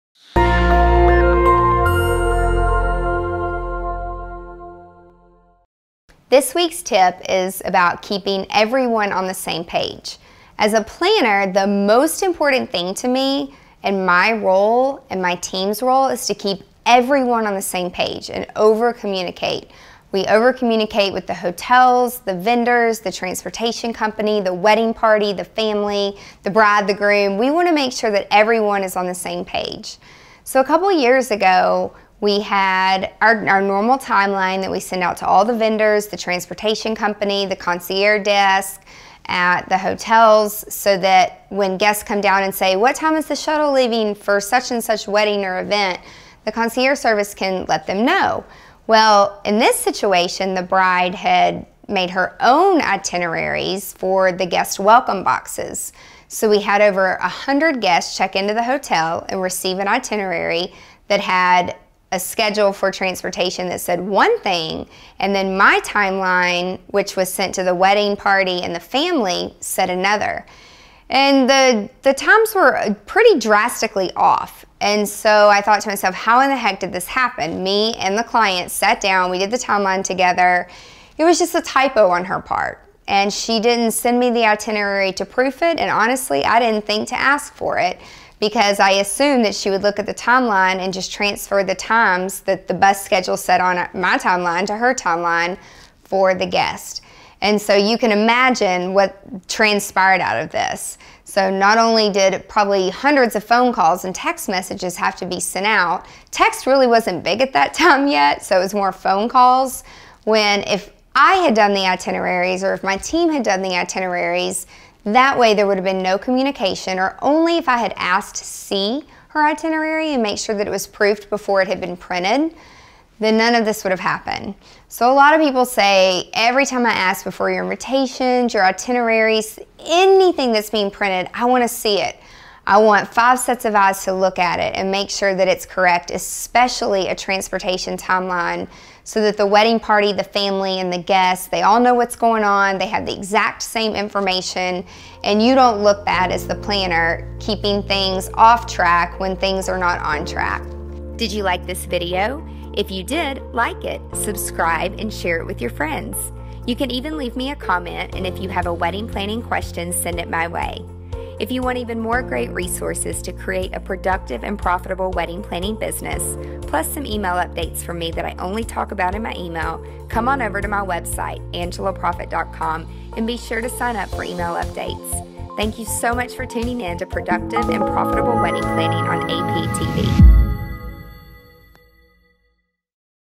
Over Communicate with Wedding Team
Over communication is my love language as a planner. Well, one time, a bride of mine decided to make her own itineraries for her guest welcome boxes. Turns out these guests received a different schedule than what I had given to the wedding party and family. It was a typo on the bride’s part, but what transpired out of that was a communication nightmare trying to get in touch with everyone. Watch today’s video tip to find out what happened!